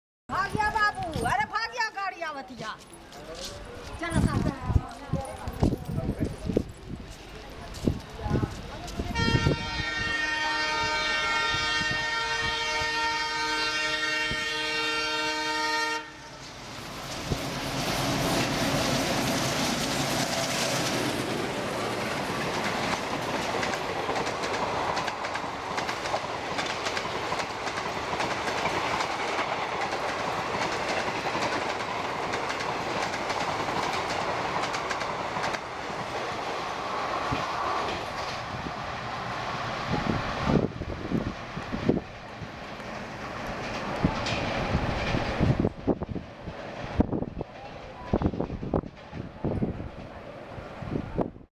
Raging Malda Town ALCo with high speed Karmabhoomi Superfast Express shatters Maghar
12408 Karmabhoomi Superfast Express (Amritsar-New Jalpaiguri) with raging WDM3A from Malda Town skips away Maghar at full pace.